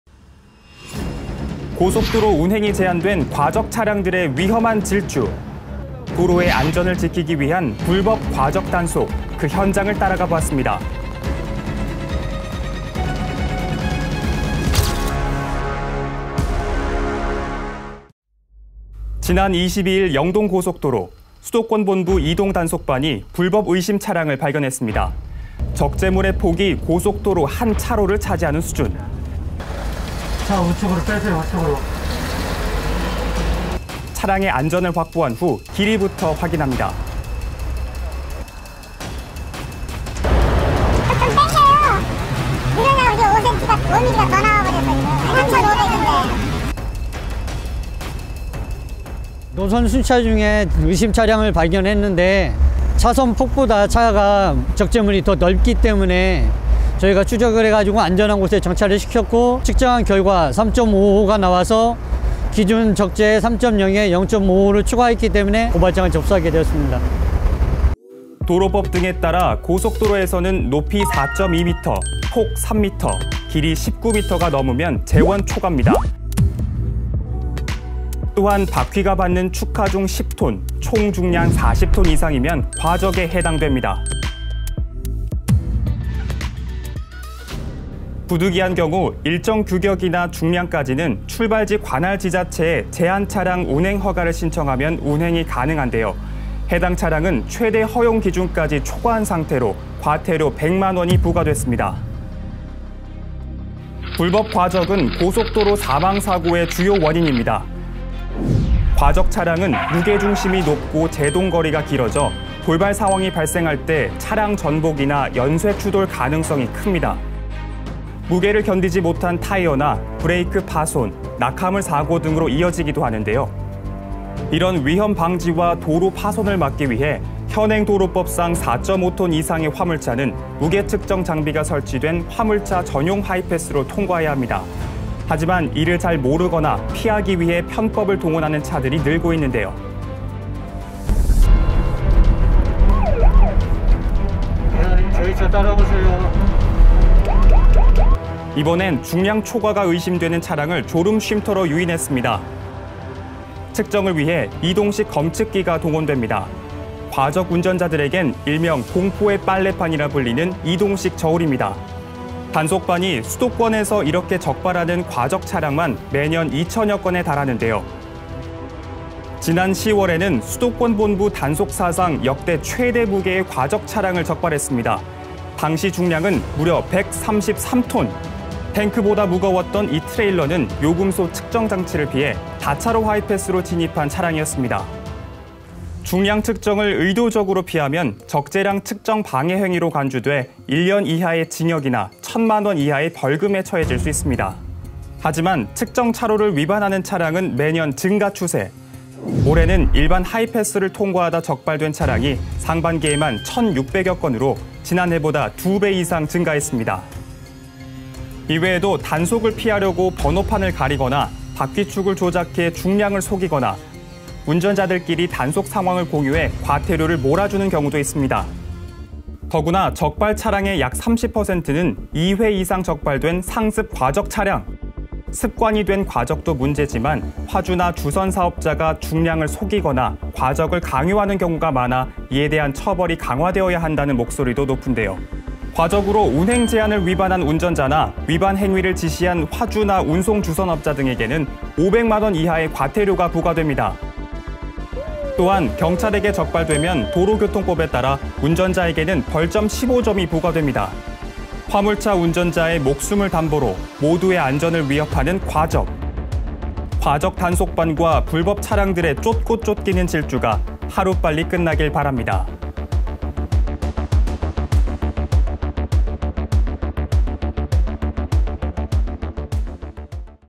[exTV가 살려드림] 단속반 레이더에 딱 걸린 과적 화물차에게 생긴 일은?
과태료 100만 원짜리 운행 제한 위반부터
탱크보다 무거운 133톤짜리 과적 차량까지
뭐든지 불법이면 다들 멈춰! 이러다 다 죽어
불법 과적 차량을 쫓는 고속도로 이동 단속반을 따라가봤습니다